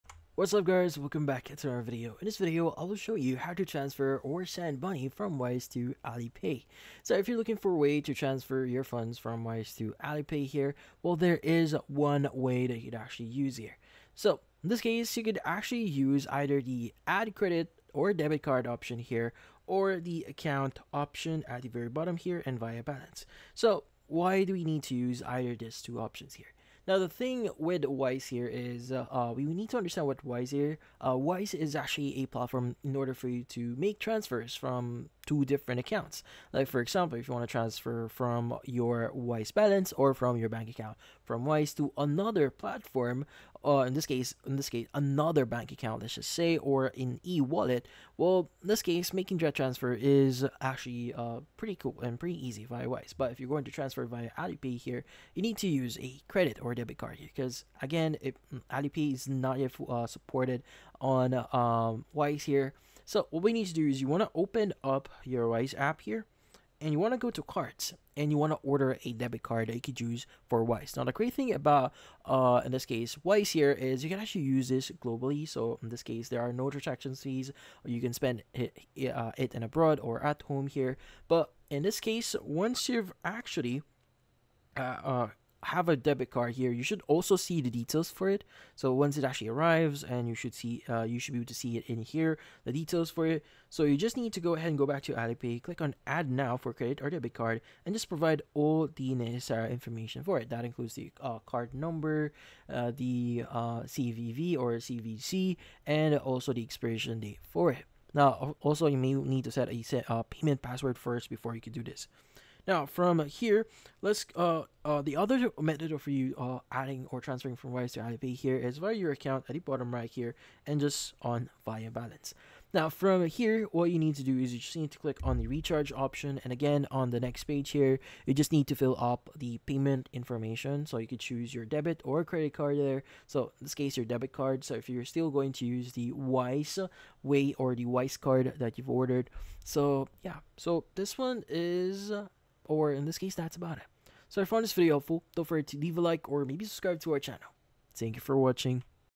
How To Transfer/ Send Money From Wise To Alipay
How do I transfer money from Wise to Alipay? In this video tutorial, I’ll show you how to send money from your Wise account directly to an Alipay wallet in China. Enjoy!